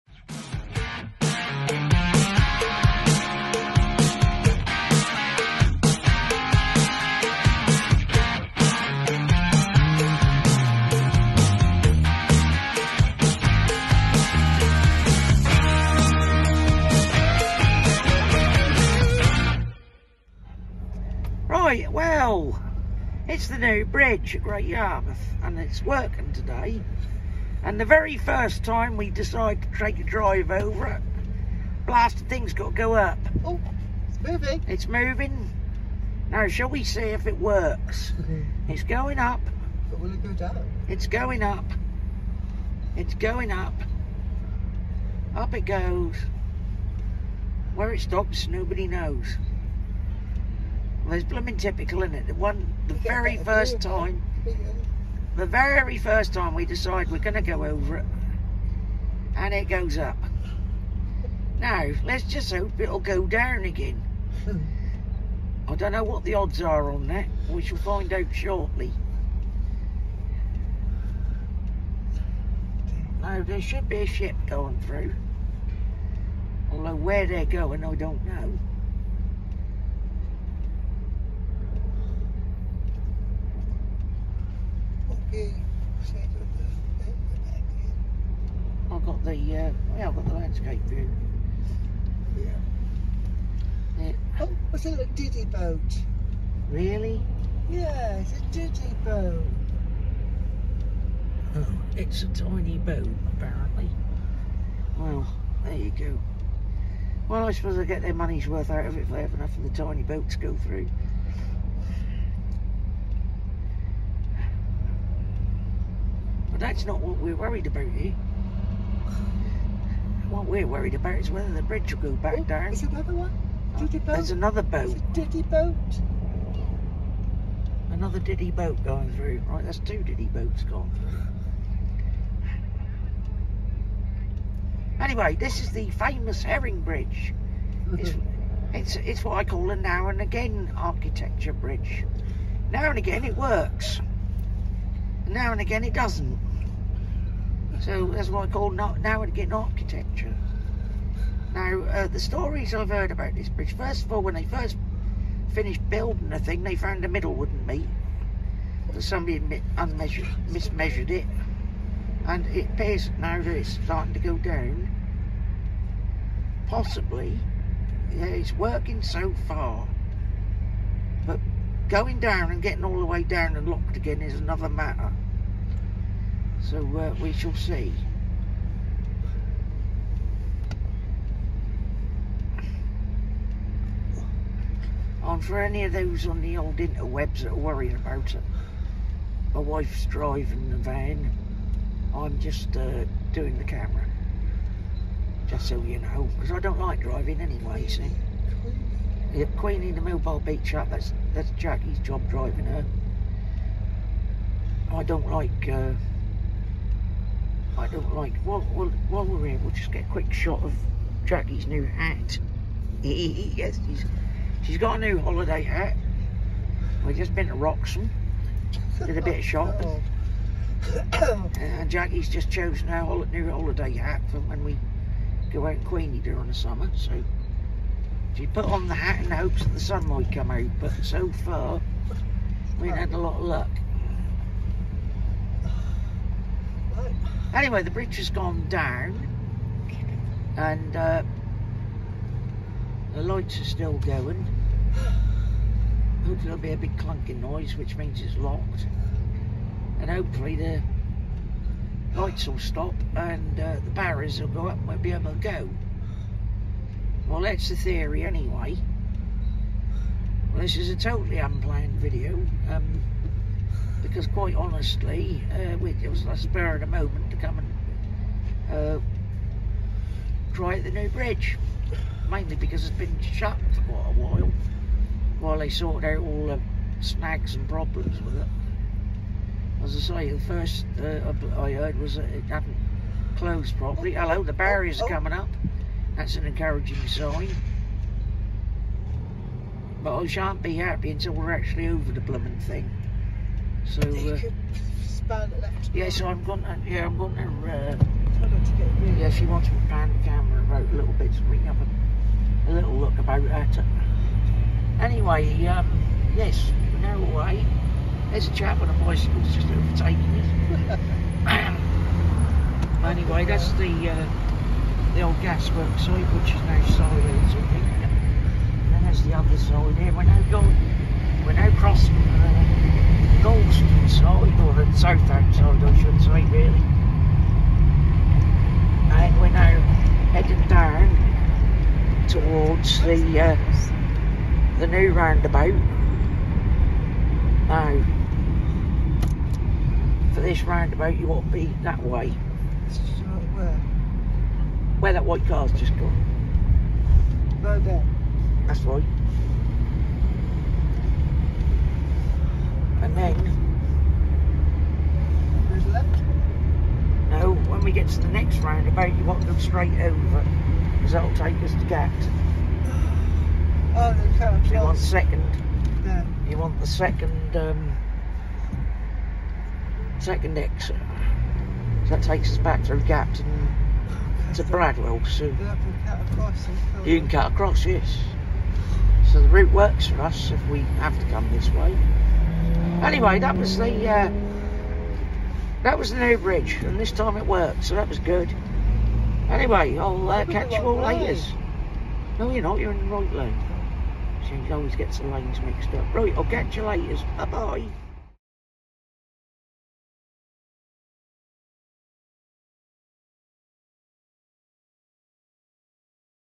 Something a bit different!
Checking out the new Herring Bridge over the river Yare at Great Yarmouth! Will it work? Who knows? Plus Mrs Thunderbolt’s new hat!